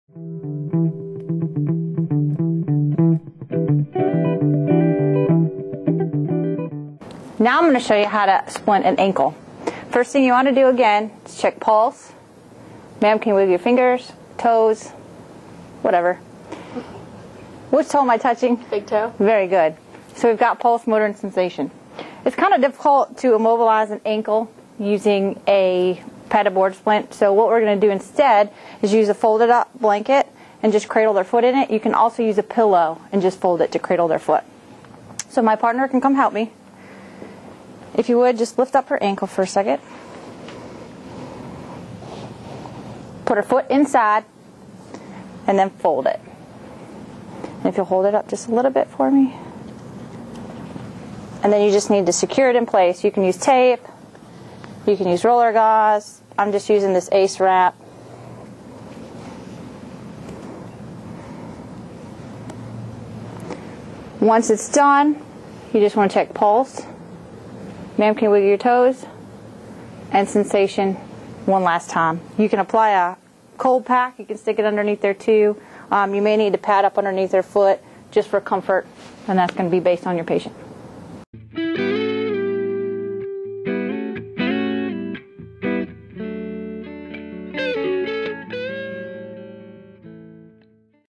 Ankle Splint
Splinting: How to splint an ankle fracture.